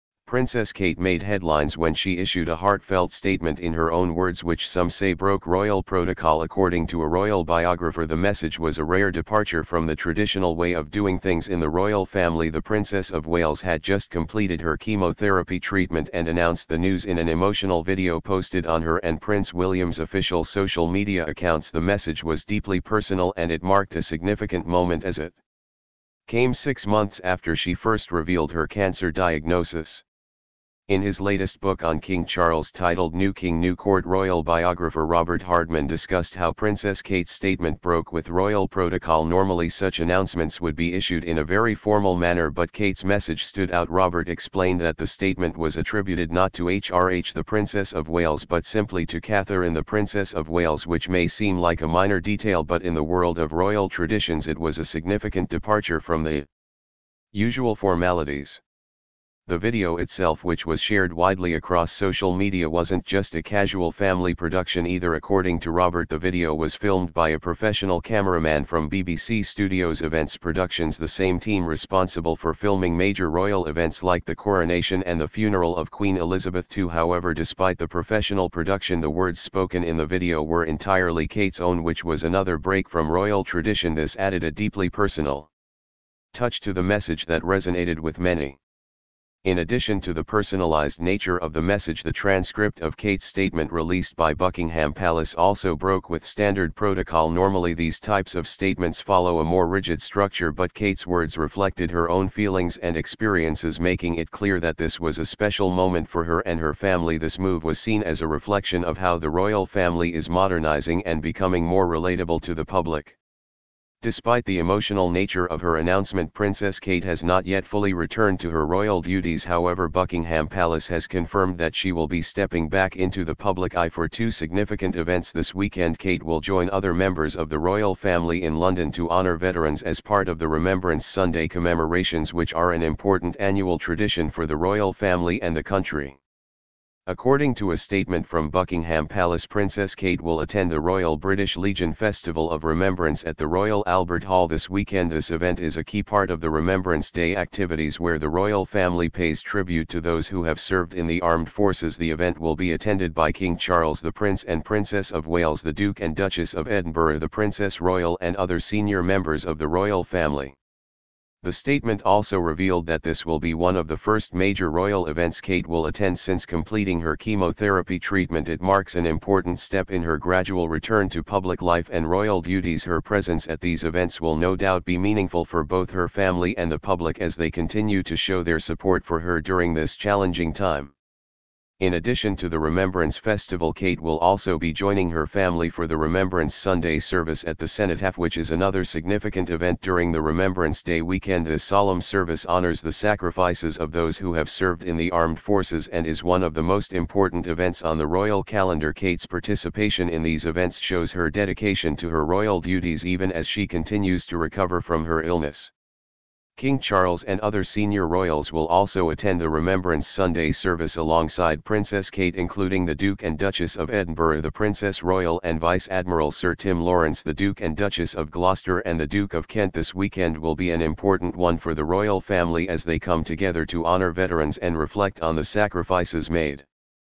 Princess Kate 'broke royal protocol' as she issued cancer statement
In this RFU Royal Family Update, Princess Kate has caused a stir by allegedly breaking royal protocol with her heartfelt cancer statement, a move that has captured the attention of royal fans around the world. The royal family is known for its adherence to tradition, but Princess Kate’s bold stance on such an important issue has raised eyebrows within the British royal family and the royalty fam. This major royal news is making headlines across the UK news and globally, as it signals a shift in the way the British monarchy approaches public statements.